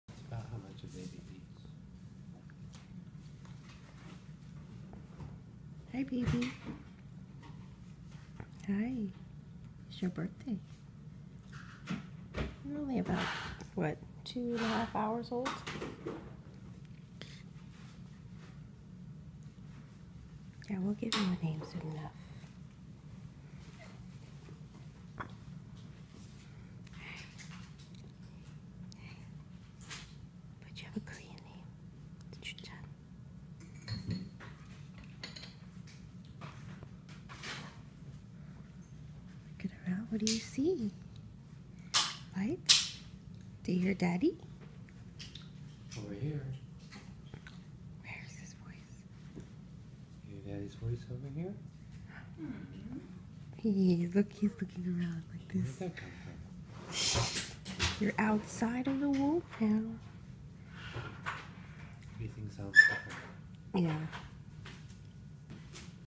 Welcome baby Noah (day 1)
Noah exploring the world outside the womb.